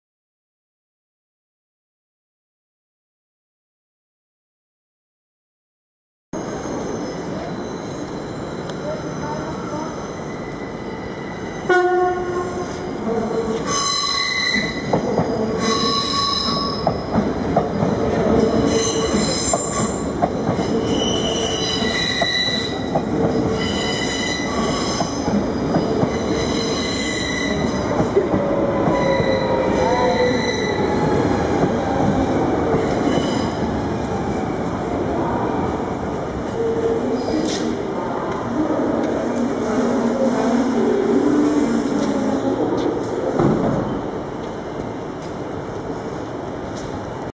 VIRM 9524@9504, aankomst, omroep, Rotterdam Centraal 7-2-2012
De voormalige OV-chipknip VIRM, jammer dat die mooie kleurencombinatie voorbij is. Wel een leuk toetertje.